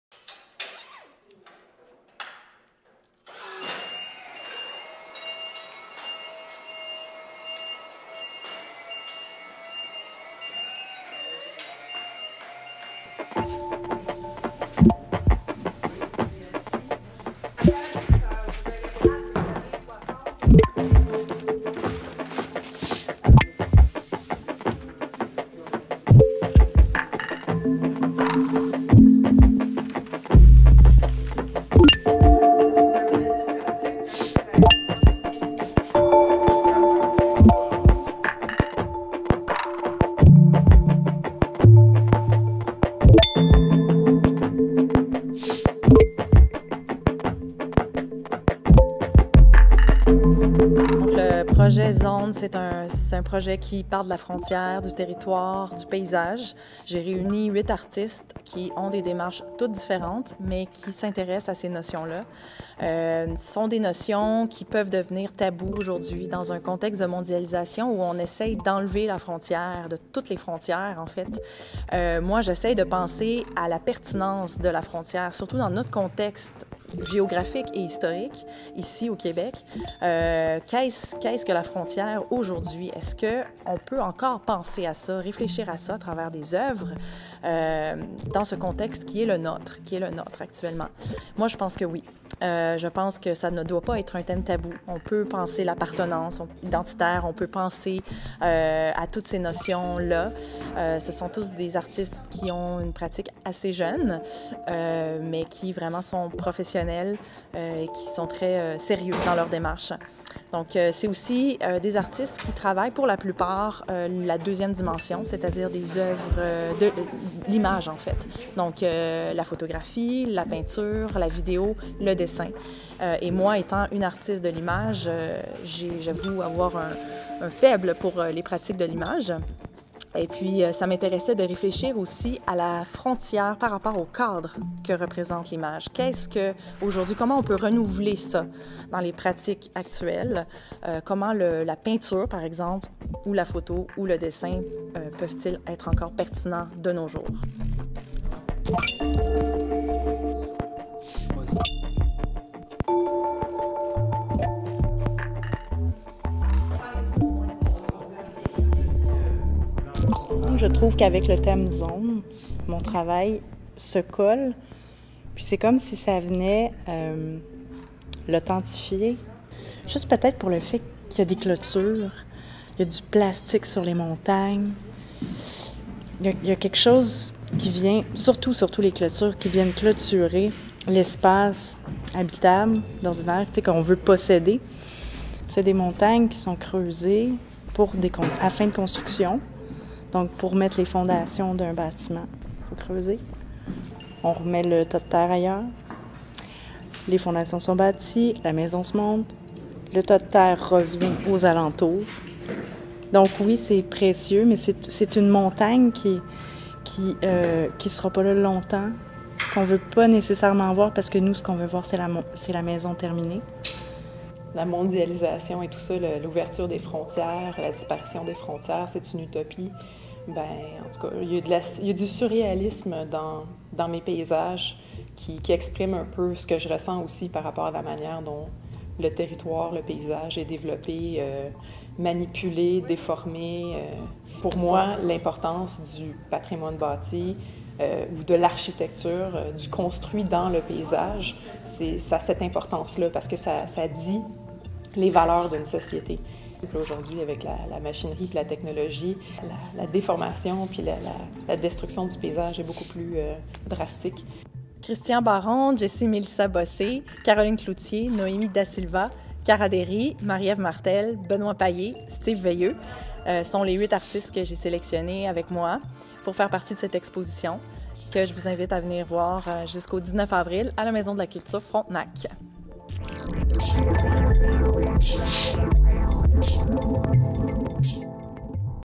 Exposition collective «Zones!» - Commissaire: Stéphanie Chalut
Depuis une quinzaine d'années, nous assistons à un retour en force de la représentation et du cadre traditionnel de l'image chez tout un pan de la jeune génération en arts visuels. L'exposition collective Zones ! montre un panorama ciblé de ces pratiques à travers les thèmes de la frontière et du territoire. Dans un contexte de mondialisation, d'immédiateté, d'instantanéité, de simultanéité et d'urgence, qu'en est-il de l'enracinement à un lieu, à l'appartenance à un territoire, qu'en est-il de la lenteur souvent créatrice de rapports durables entre les êtres? Comment la frontière peut t-elle encore participer à civiliser notre humanité à travers une identité nationale donnée? Zones ! affirme le nécessaire besoin d'enracinement de l'homme dans un territoire géographique. Avec les œuvres de Stéphanie Chalut (commissaire-artiste), Christian Baron, Jessie Bossé, Caroline Cloutier, Noémie Da Silva, Cara Déry, Marie-Ève Martel, Benoit Paillé, Steve Veilleux.